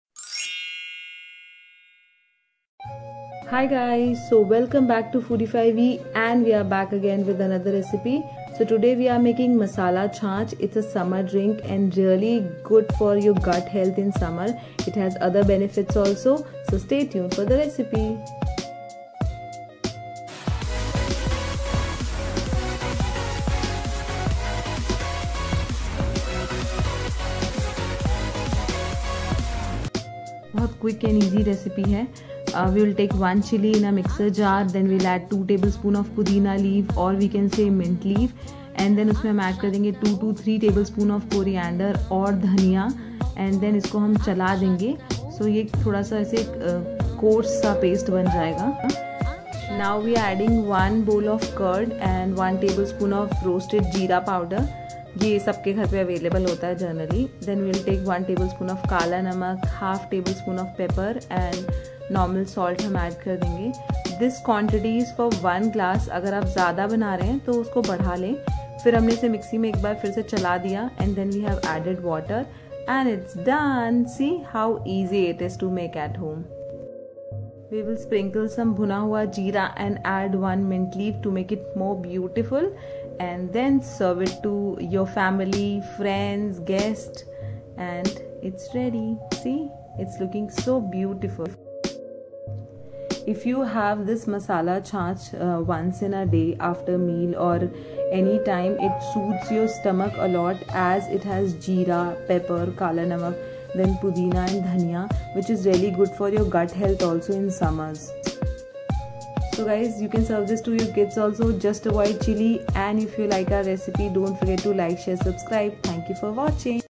Masala Chaas Recipe || Spiced buttermilk || Summer Drink || Healthy drink
Masala Chaas has its own benefits.  It great for gut health  and soothes your stomach in summer.   Its saves you from dehydration and aid in digestion. It is easy and quick to make.

Serving - 1 Glass
Ingredients
Curd- 1 bowl
Pudina/Mint- 2tbs
Coriander / dhaniya- 2-3 tbs
Chilli-1  Optional
Pepper - 1tbs
Salt- taste according
Balck Salt 1 tbs
Roasted Jeera powder- 1tbs

In a mixer add Chilli, Mint, Coriander and grind it in a coarse paste. Now add Curd, pepper, jeera/ Cumin powder, black salt and churn it in a pulse speed for 15 sec.

Add water as per you linking  and serve it.
You can add Ice cubes also.

Isn't it easy and quick!
Happy Cooking!

Musician: Jef